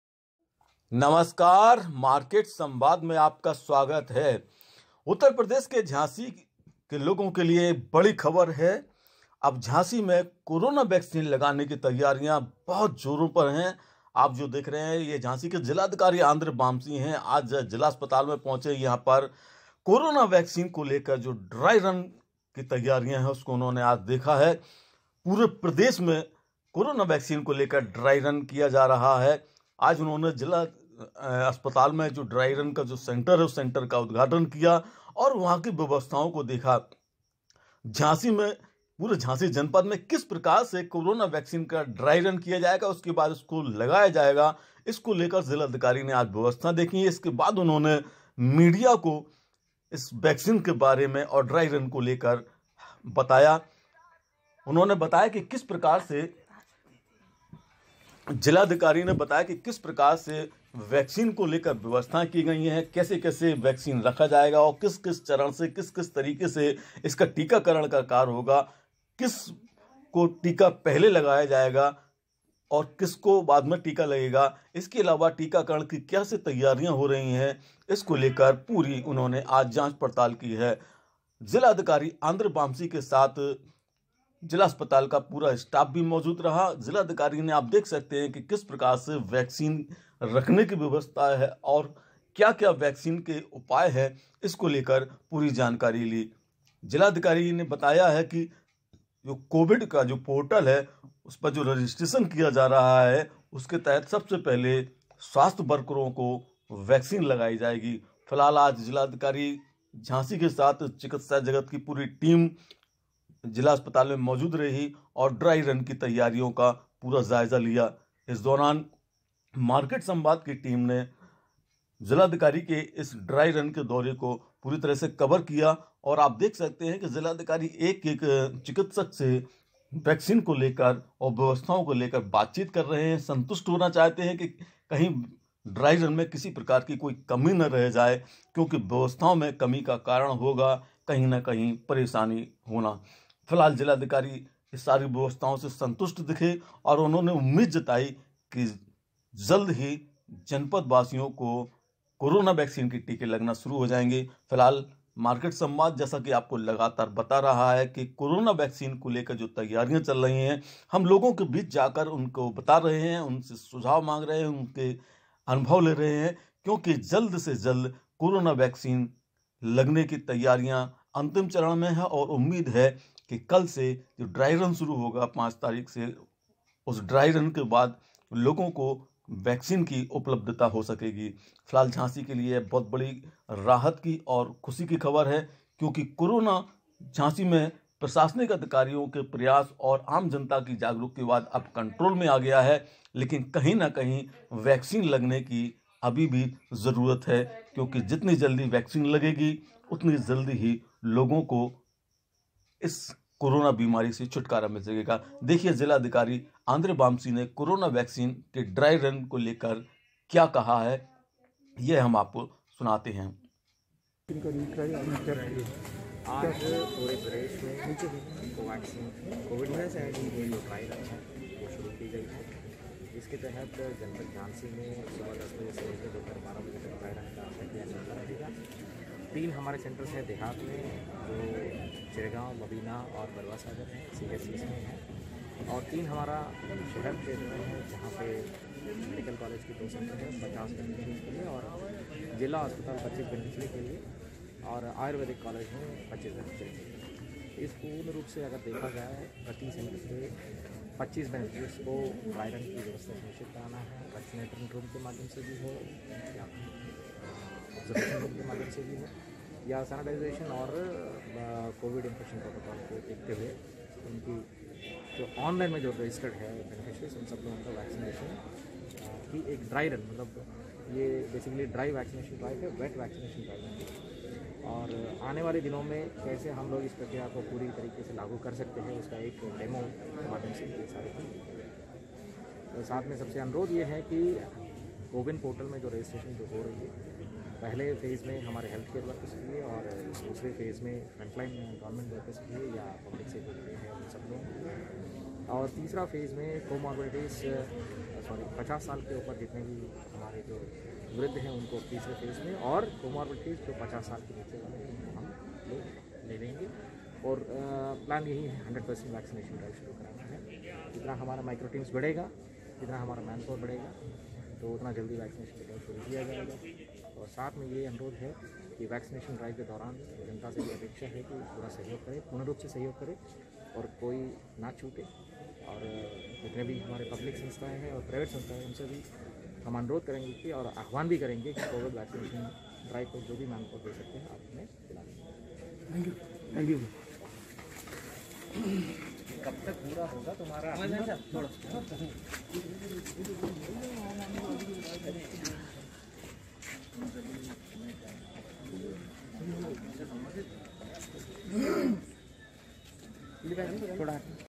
झाँसी में वैक्सीनेशन की ट्रायल की तैयारियां, देखें क्या बोले जिलाधिकारी?रिपोर्ट-अनिल मौर्य
झांसी के जिलाधिकारी ने आज कोरोना वैक्सीन के  ड्राई रन की की तैयारियों को लेकर आज जिला अस्पताल में दौरा किया उन्होंने  तैयारियों को देखा इसके साथ ही दिशा निर्देश दिए इसके अलावा जिलाधिकारी ने मीडिया से बातचीत में बताया कि किस प्रकार से झांसी जनपद में कोरोना वैक्सीन के लगाने  का कार्य कहां कहां और किस तरह से होगा